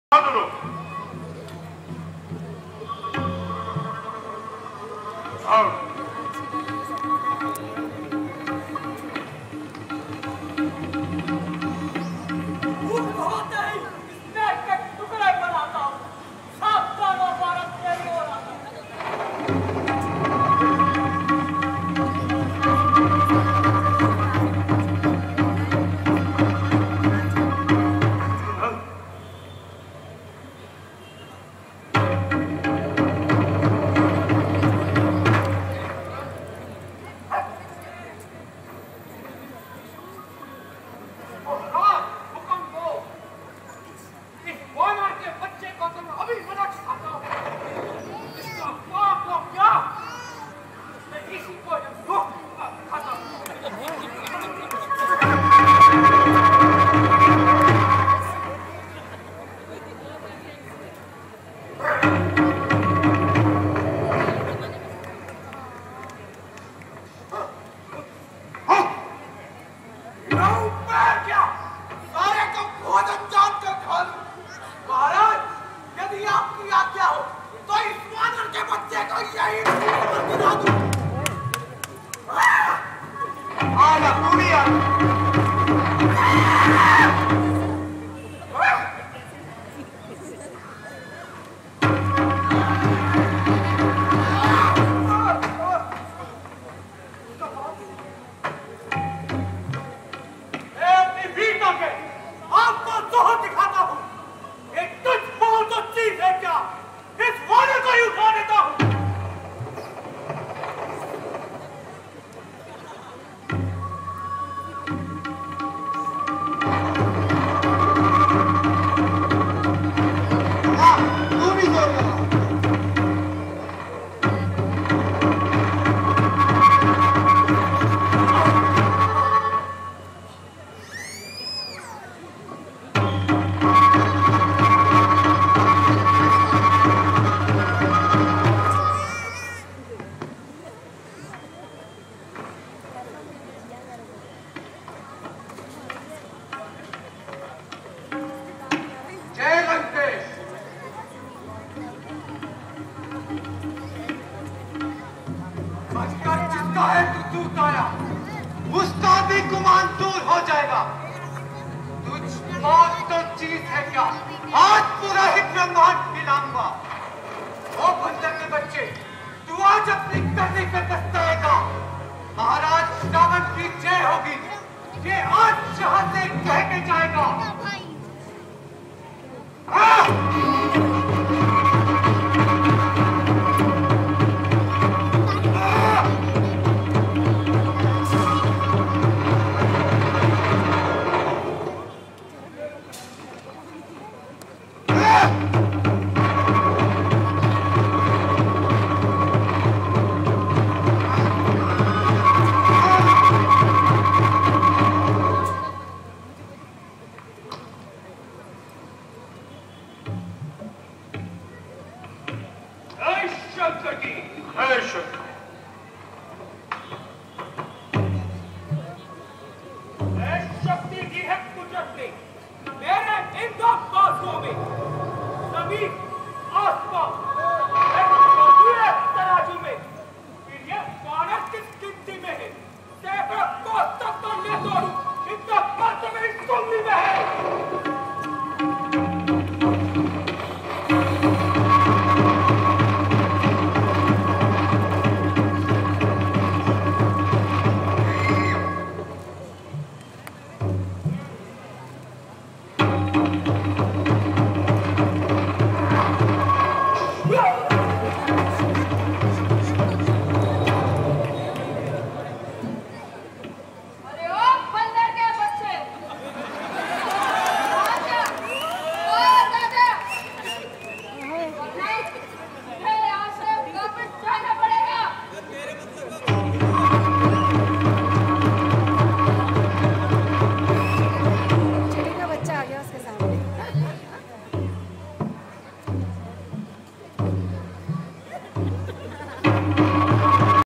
Ramleela
The greatest hindu epic...being shown for good of society by ameture artists like you and me..taking time out from there busy lives...